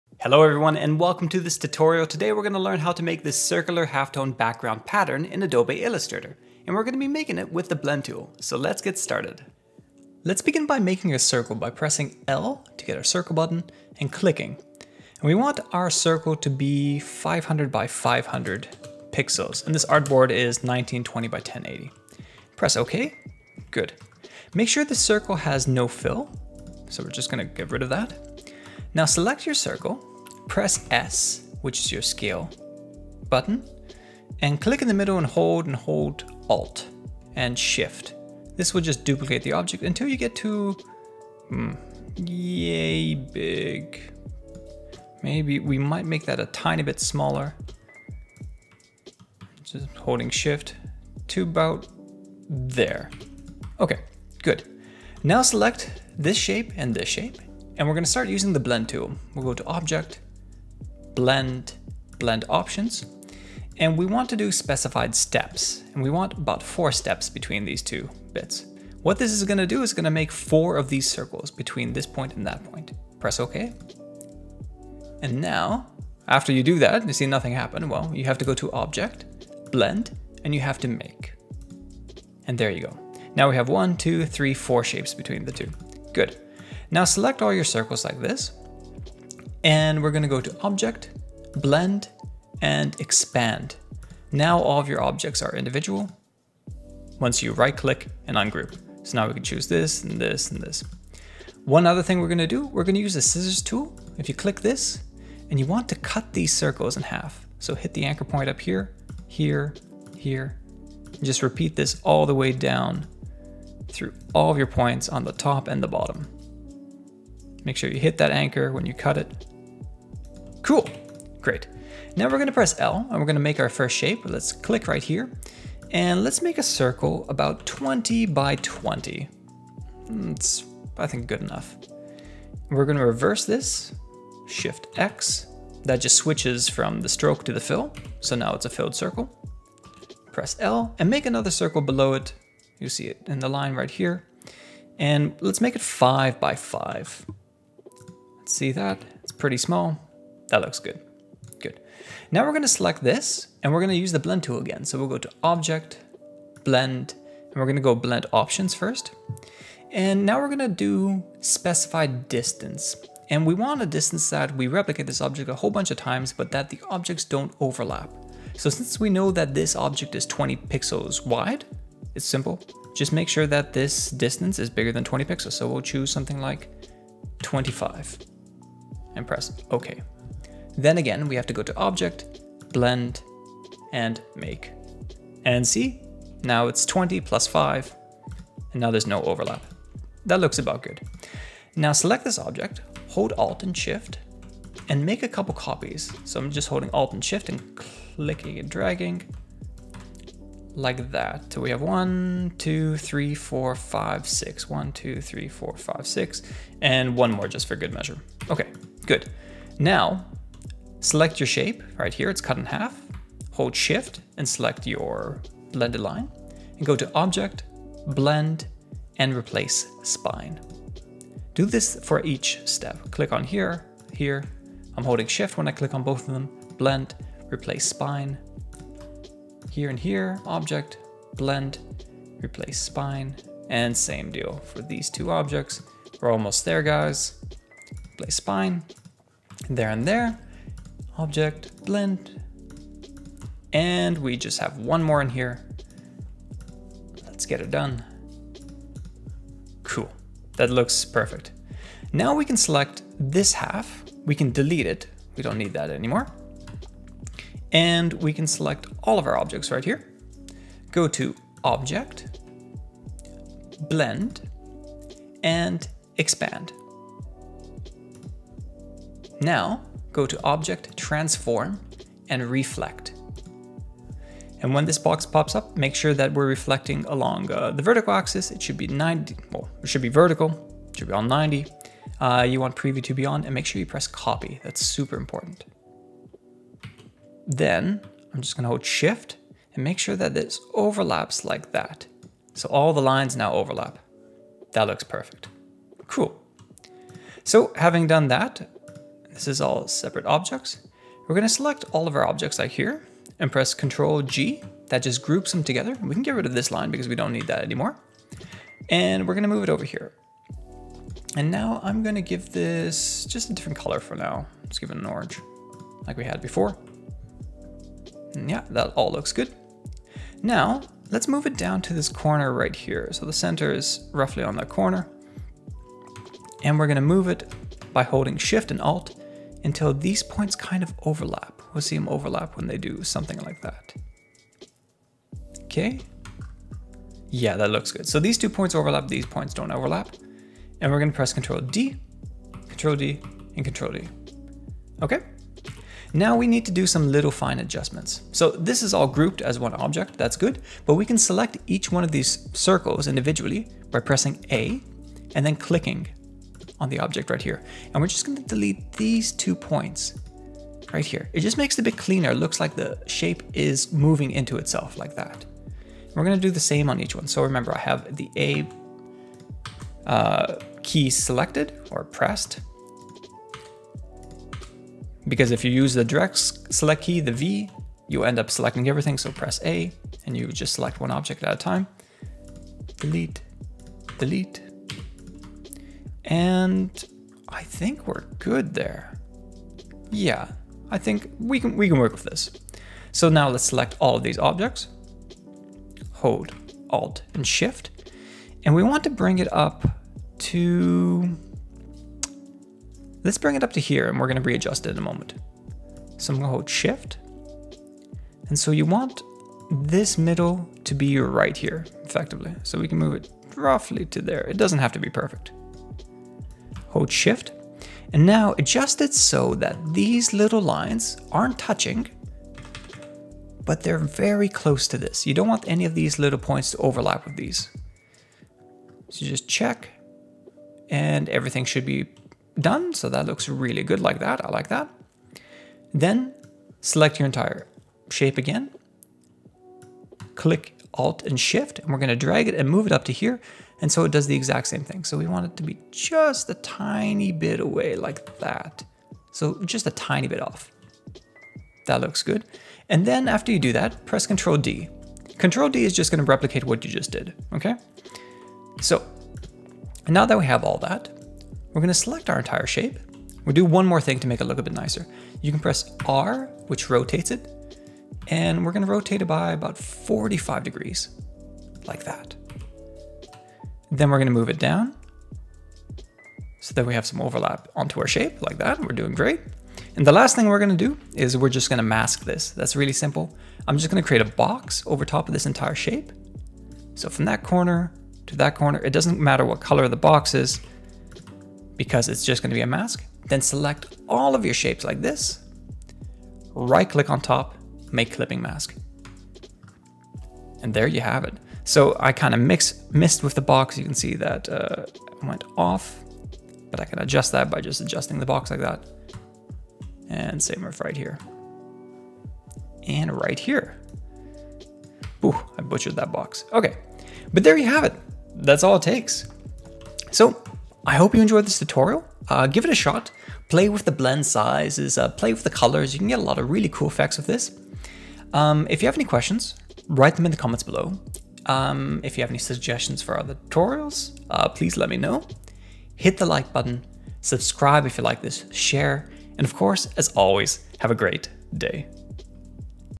Circular Halftone Pattern - Adobe Illustrator Tutorial
Come learn how to make this circular halftone pattern in Adobe Illustrator. Actually it doesn't need to be circles even! Make it with squares, make it with fish, make it with lines; the technique is the same.

In this tutorial, you will learn to use the:
- Move tool
- Duplicate tool
- Blend tool

You will have to modify this shape a little bit in order for it to work properly and look seamless, but at least no math is involved.
This technique will work in Adobe Illustrator CC and Illustrator CS6 and below.